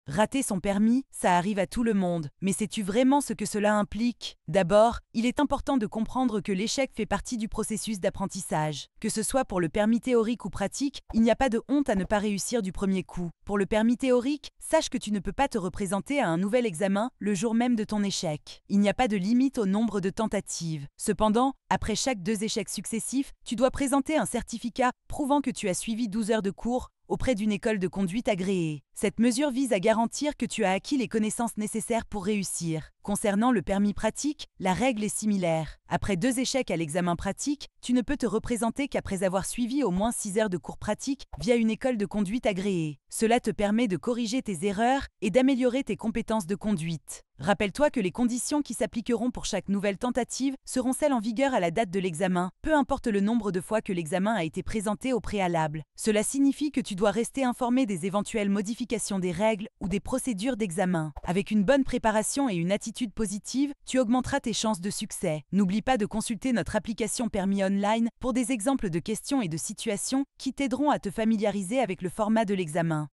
𝐄𝐬𝐭-𝐜𝐞 𝐠𝐫𝐚𝐯𝐞 𝐝𝐞 𝐫𝐚𝐭𝐞𝐫 𝐬𝐨𝐧 𝐩𝐞𝐫𝐦𝐢𝐬 𝐩𝐥𝐮𝐬𝐢𝐞𝐮𝐫𝐬 𝐟𝐨𝐢𝐬?
🚗💡 Raté ton permis plus d'une fois? Découvre pourquoi ce n'est pas la fin du monde et comment te préparer pour la prochaine fois! 📘🚦

𝐂𝐨𝐦𝐩𝐫𝐞𝐧𝐝𝐫𝐞 𝐥'É𝐜𝐡𝐞𝐜 𝐝𝐚𝐧𝐬 𝐥𝐞 𝐏𝐫𝐨𝐜𝐞𝐬𝐬𝐮𝐬 𝐝'𝐎𝐛𝐭𝐞𝐧𝐭𝐢𝐨𝐧 𝐝𝐮 𝐏𝐞𝐫𝐦𝐢𝐬 𝐝𝐞 𝐂𝐨𝐧𝐝𝐮𝐢𝐫𝐞:
Rater son permis de conduire est une étape normale dans le processus d'apprentissage. Que ce soit pour le permis théorique ou pratique, il est essentiel de reconnaître qu'il n'y a pas de honte à échouer lors de la première tentative. Cet échec fait partie intégrante du parcours vers la réussite.

𝐑è𝐠𝐥𝐞𝐬 𝐀𝐩𝐫è𝐬 É𝐜𝐡𝐞𝐜 𝐚𝐮 𝐏𝐞𝐫𝐦𝐢𝐬 𝐓𝐡é𝐨𝐫𝐢𝐪𝐮𝐞:
Pour le permis théorique, il est important de savoir qu'après un échec, il n'est pas possible de se représenter le jour même. Il n'existe pas de limite au nombre de tentatives, mais après deux échecs consécutifs, il est nécessaire de présenter un certificat de 12 heures de cours suivis dans une école de conduite agréée. Cette mesure assure que le candidat a acquis les connaissances requises pour réussir.

𝐏𝐫𝐨𝐜é𝐝𝐮𝐫𝐞 à 𝐒𝐮𝐢𝐯𝐫𝐞 𝐀𝐩𝐫è𝐬 É𝐜𝐡𝐞𝐜 𝐚𝐮 𝐏𝐞𝐫𝐦𝐢𝐬 𝐏𝐫𝐚𝐭𝐢𝐪𝐮𝐞:
En ce qui concerne le permis pratique, les règles sont similaires. Après deux échecs, le candidat doit suivre au moins 6 heures de cours pratiques dans une école de conduite agréée avant de se représenter. Ces cours sont cruciaux pour corriger les erreurs et améliorer les compétences de conduite.

𝐈𝐦𝐩𝐨𝐫𝐭𝐚𝐧𝐜𝐞 𝐝𝐞 𝐑𝐞𝐬𝐭𝐞𝐫 𝐈𝐧𝐟𝐨𝐫𝐦é 𝐝𝐞𝐬 𝐑è𝐠𝐥𝐞𝐬 𝐞𝐭 𝐏𝐫𝐨𝐜é𝐝𝐮𝐫𝐞𝐬:
Il est crucial de se tenir informé des règles et procédures d'examen, car les conditions applicables à chaque nouvelle tentative dépendent des réglementations en vigueur à la date de l'examen. Les candidats doivent donc rester à jour sur d'éventuelles modifications des règles ou des procédures.

𝐏𝐫é𝐩𝐚𝐫𝐚𝐭𝐢𝐨𝐧 𝐞𝐭 𝐑𝐞𝐬𝐬𝐨𝐮𝐫𝐜𝐞𝐬 𝐩𝐨𝐮𝐫 𝐑é𝐮𝐬𝐬𝐢𝐫 𝐬𝐨𝐧 𝐏𝐞𝐫𝐦𝐢𝐬:
Une bonne préparation et une attitude positive sont essentielles pour augmenter les chances de succès. Il est recommandé de consulter l'application Permis Online pour accéder à des exemples de questions et de situations, ce qui aide à se familiariser avec le format de l'examen et à se préparer efficacement.